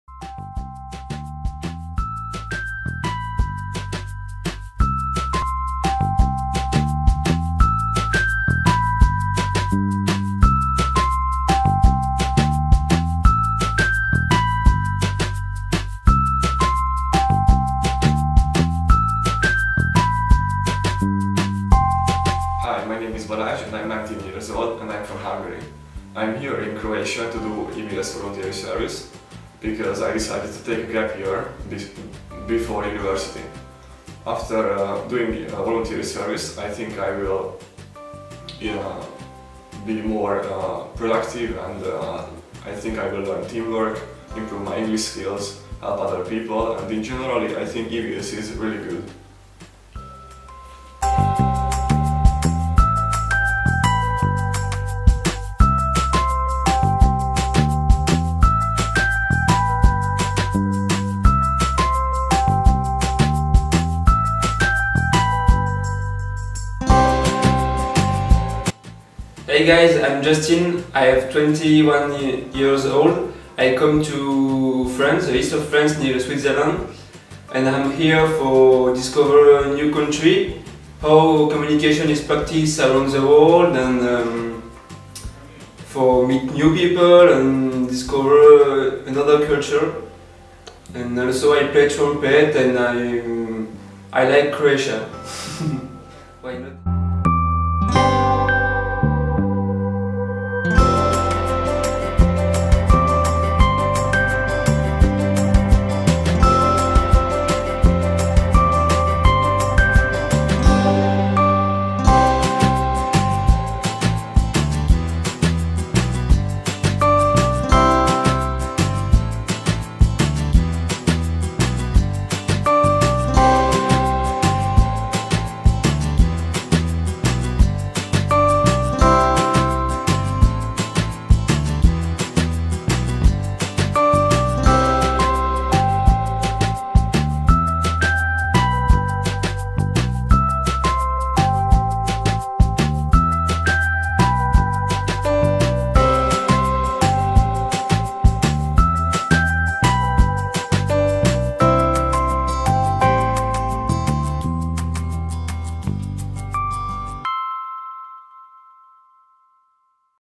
Balázs Fejes & Justin Roche EVS volunteers from Hungary & France interview 2018
Video is part of a project "International volunteer day" a short term European  Voluntary Service in Petrinja, Croatia.